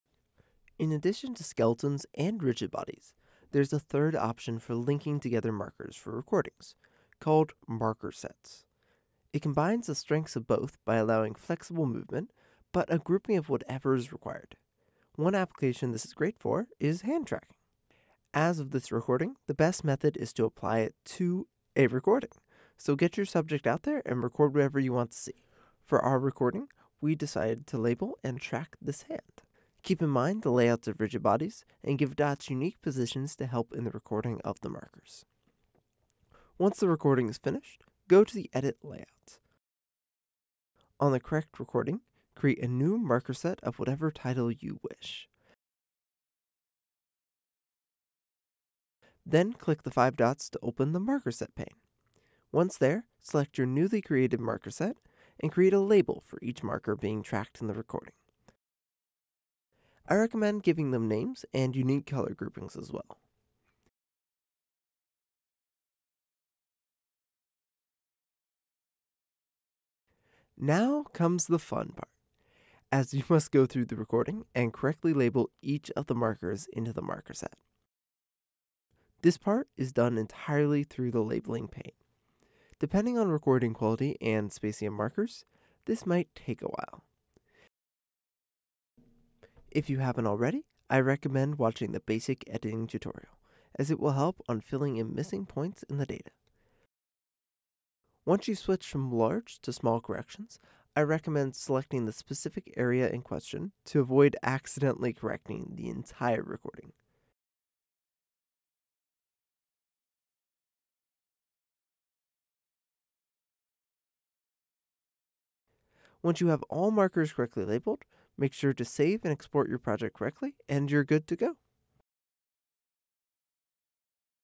Motion Capture Tutorial: Markersets
Learn the basics of markersets in Motive Body version 2.0.2 on the GRID Lab's motion capture system.

Directed and edited by: Andy Andermatt
Hand Model: Jack Sherlock